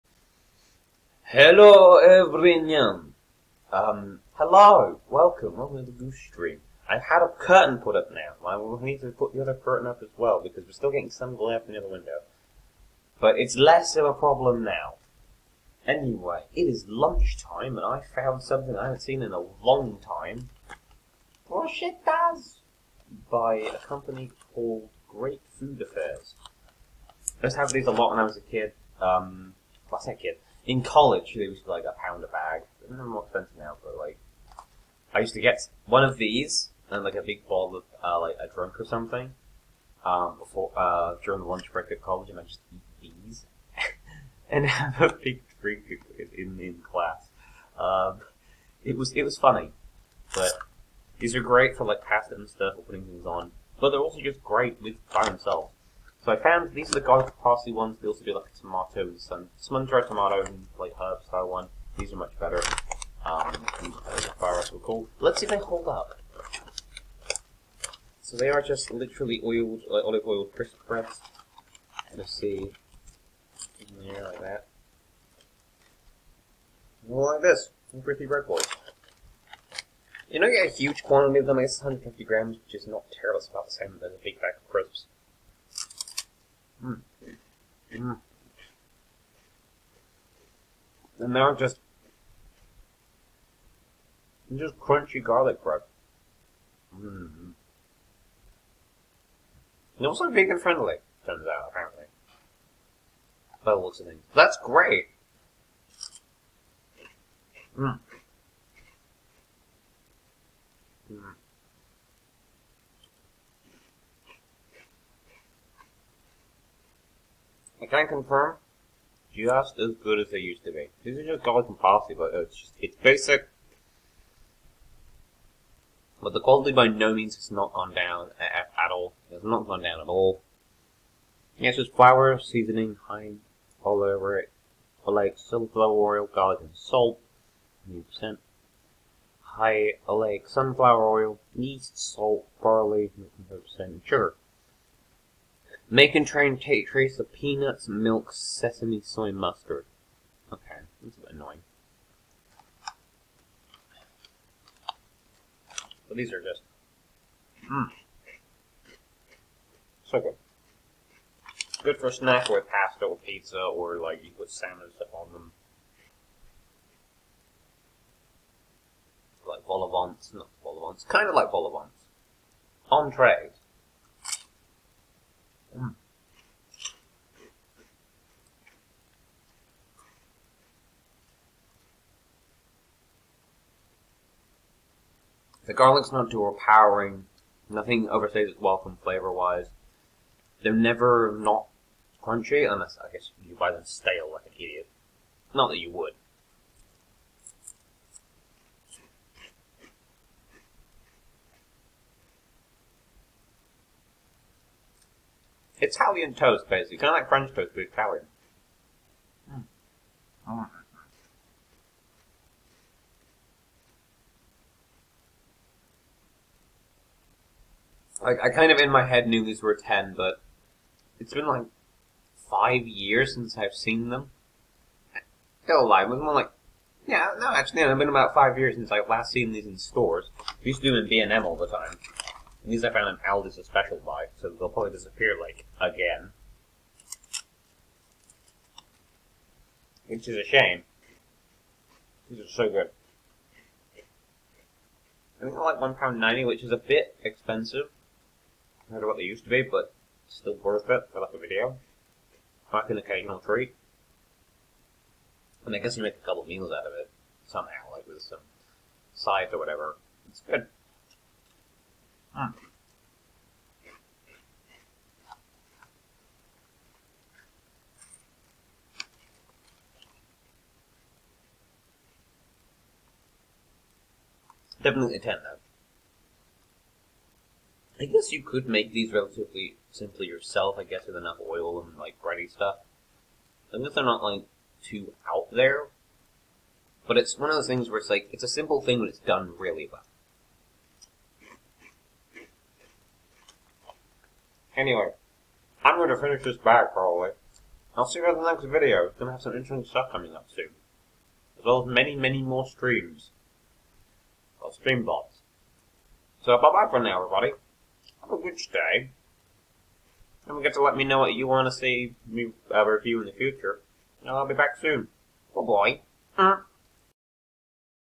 bruschetta review: garlic bread in a bag!
i used to love these as a student, lets see if they hold up to the taste test of time!